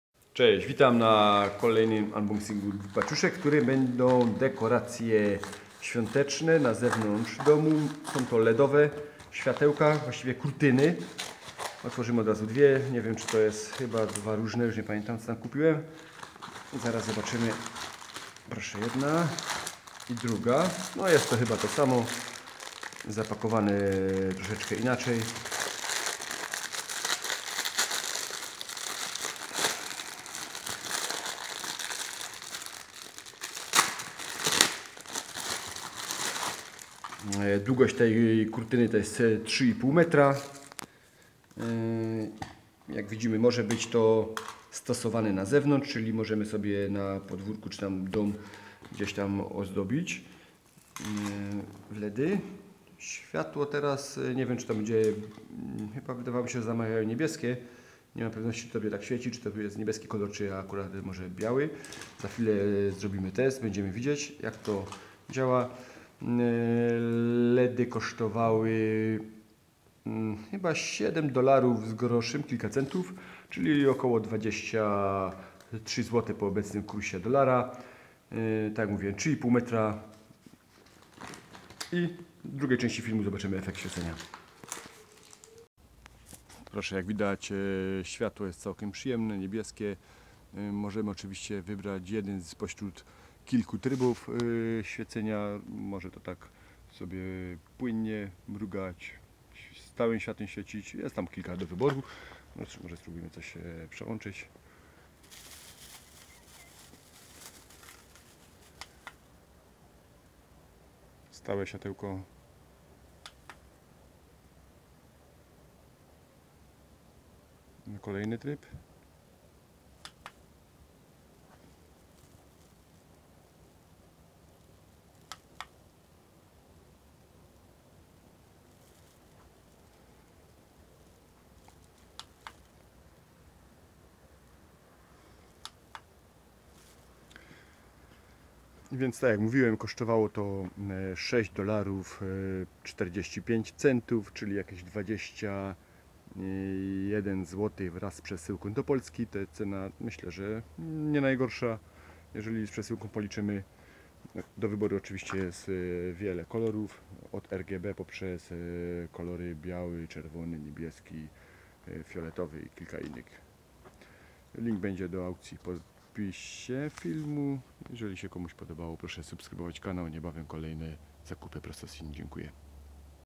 Sople kurtyna LED z Aliexpress
Sople kurtyna LED zakupiona w Chinach za 22zł z przesyłką.
3,5 metrowy odcinek z wyborem kilku trybów świecenia.
zakupione tu